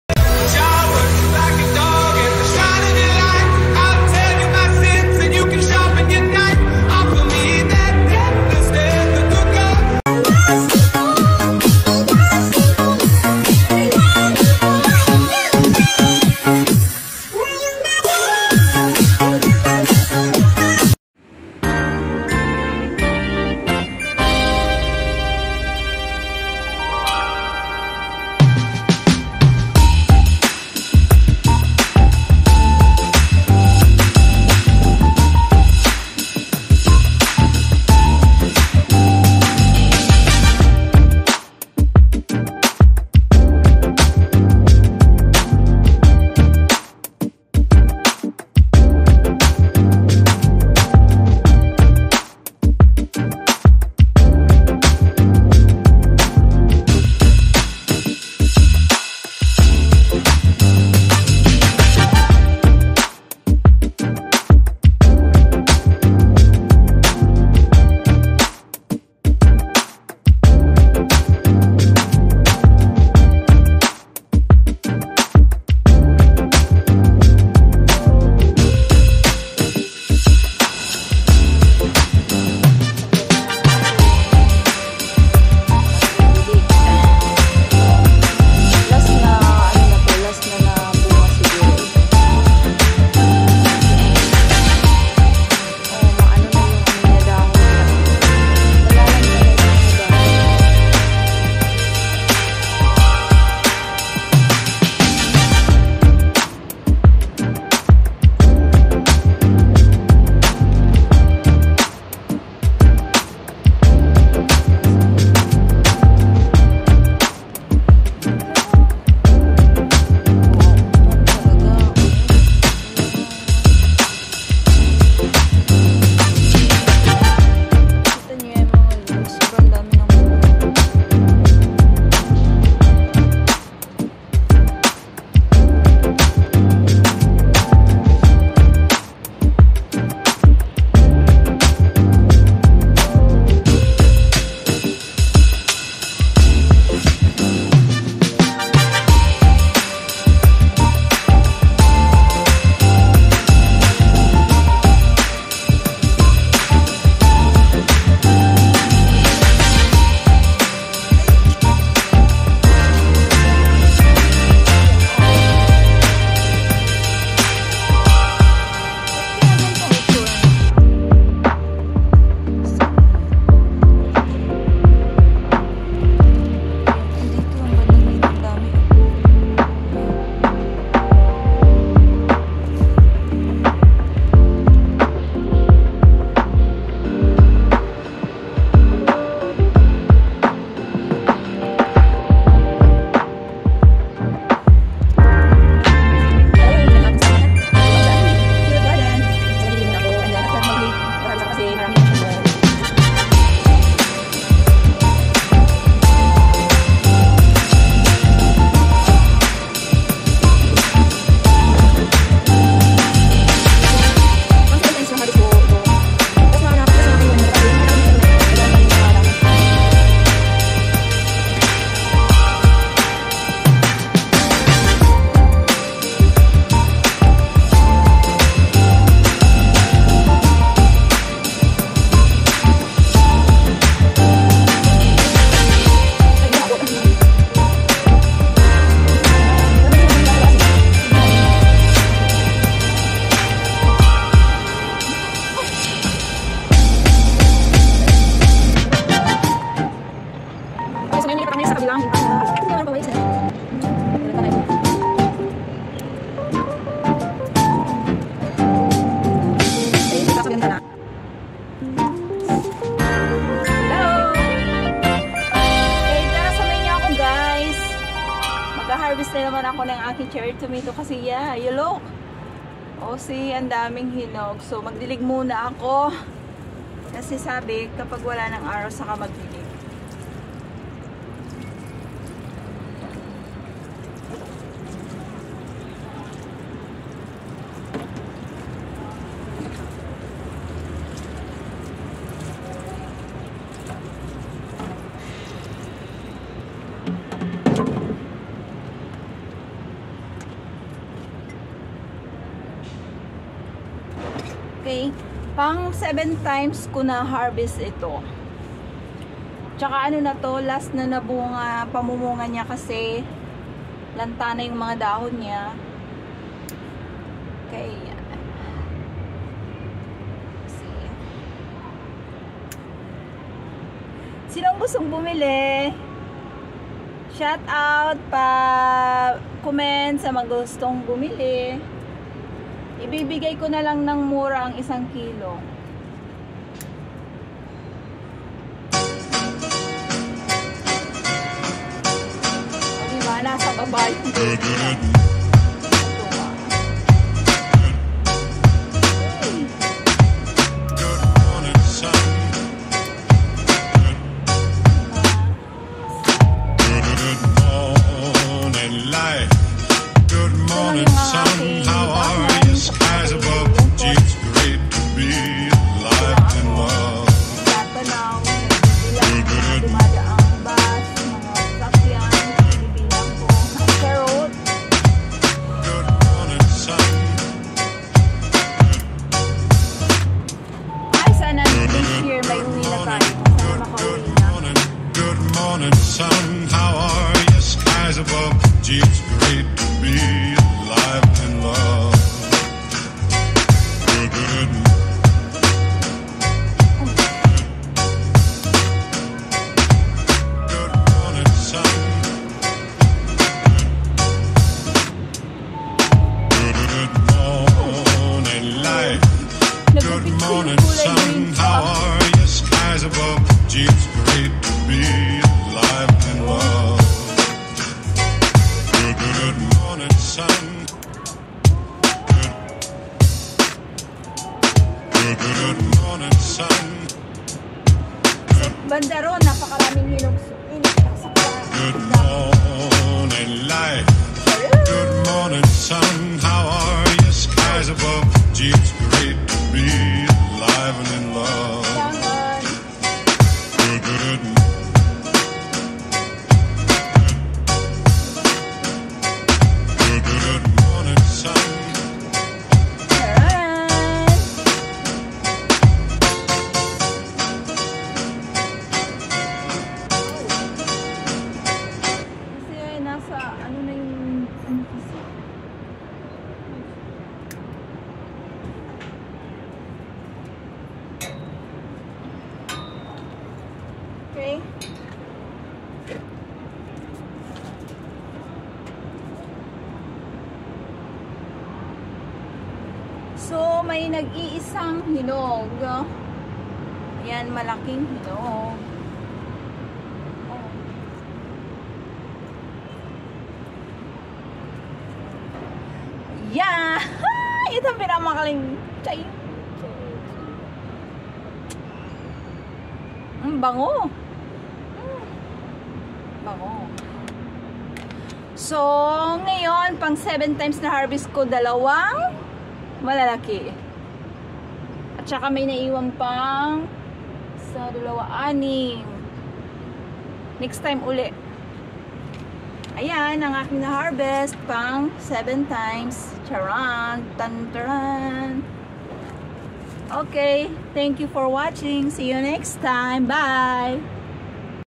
Buhay-OFW pang pitong beses na harvest Ng cherry tomatoes
Harvesting cherry tomatoes /indoor gardening